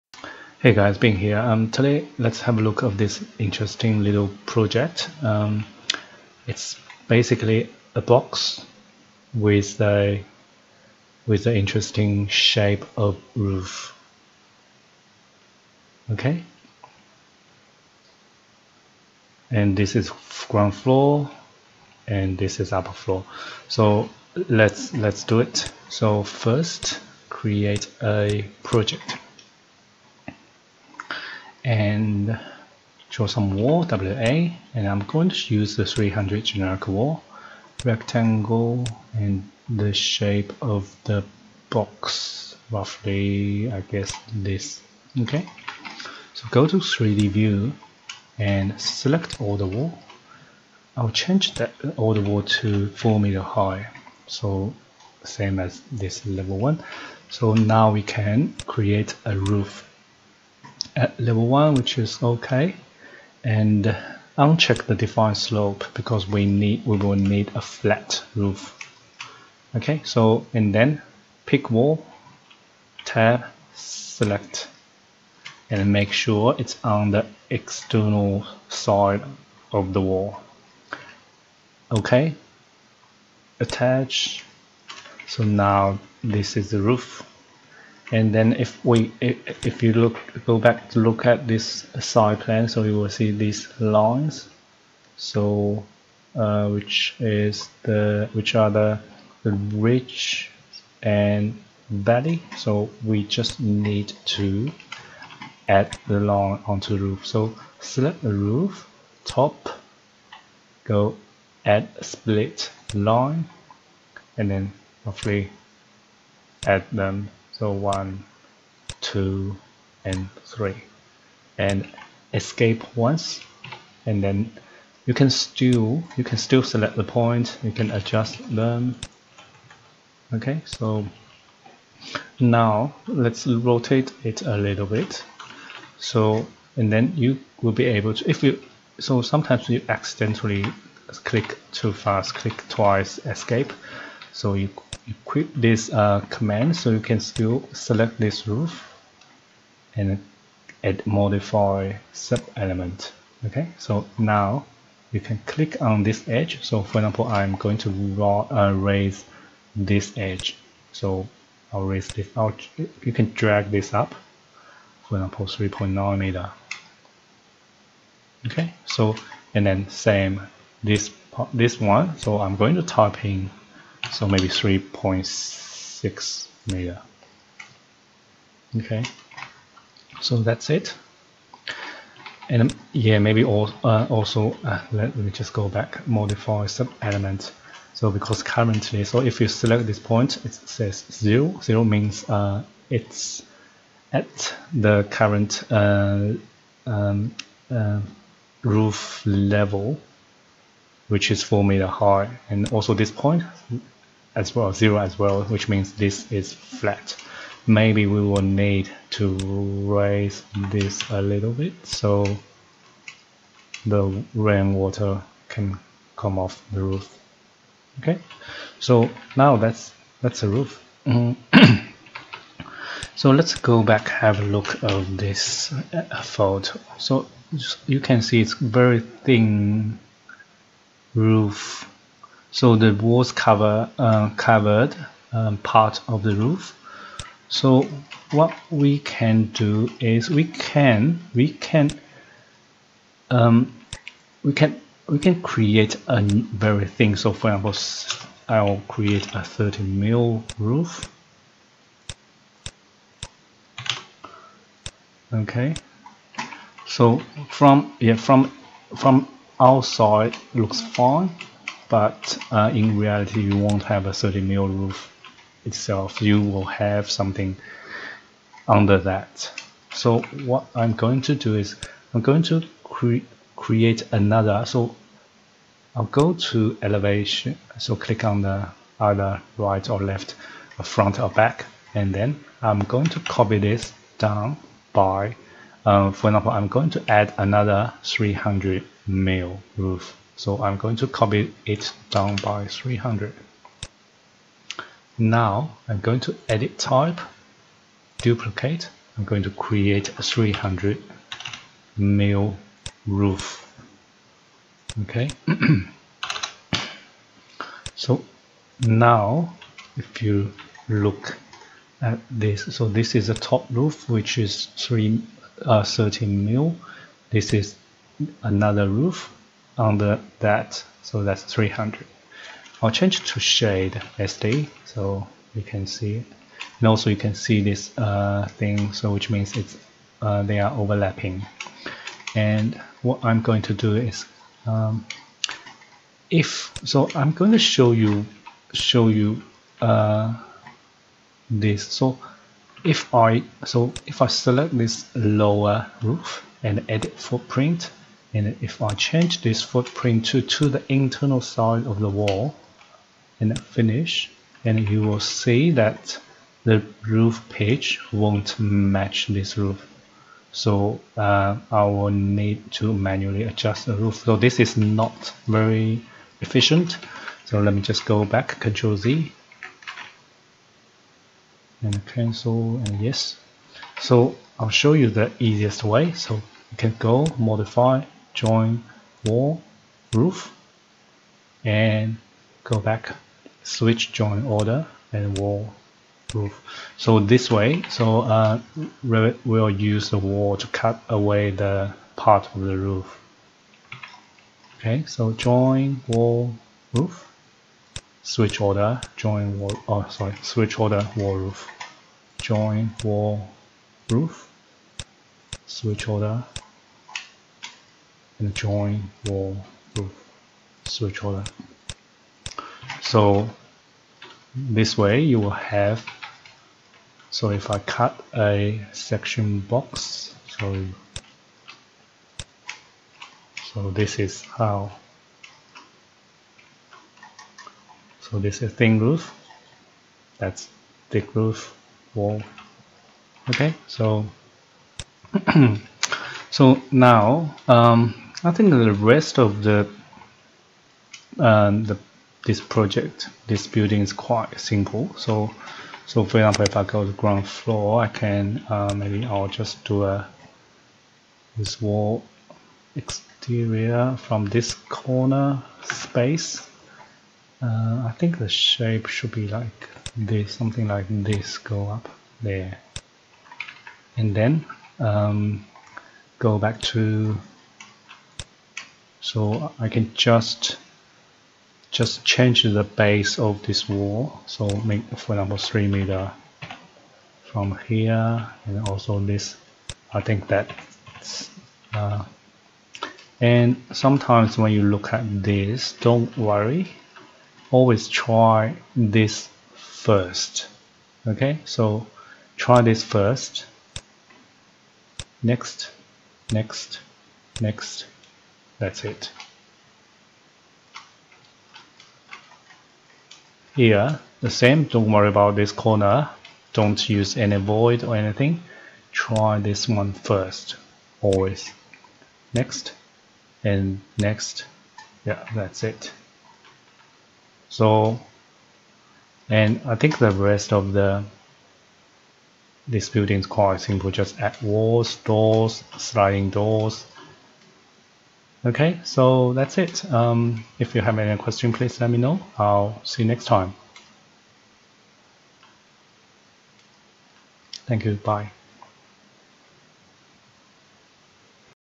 Everyday Revit (Day 306) - Simple Building Form Exercise
Today we are going to model a very simple building form, let me know if you have any questions.